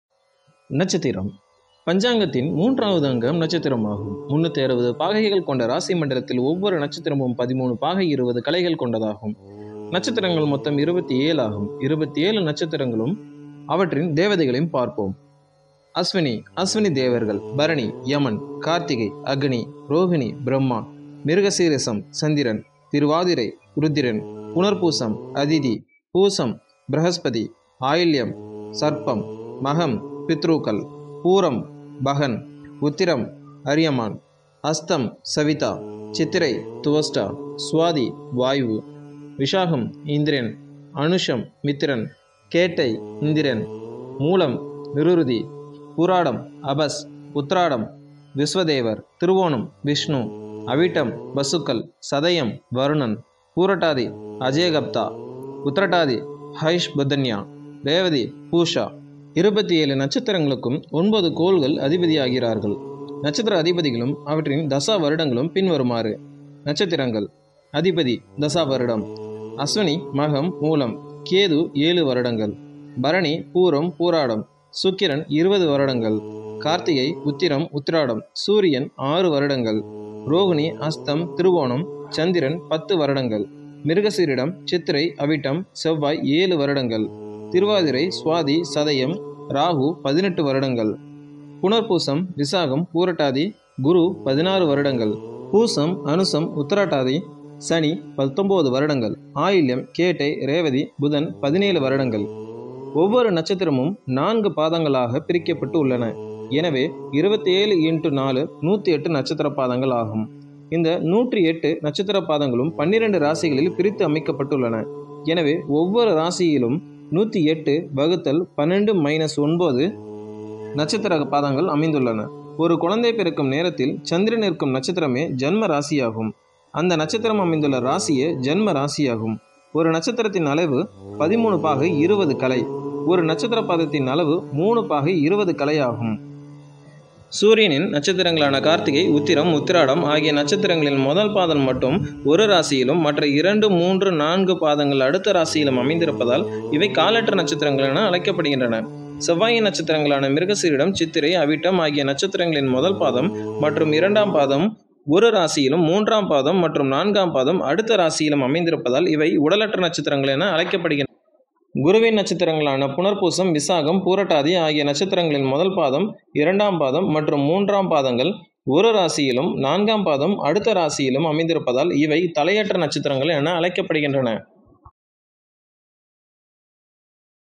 27 நட்சத்திரங்களின் தேவதை மற்றும் அதிபதி பற்றி அறிந்து கொள்ளலாம்.
This video is related about Basic Astrology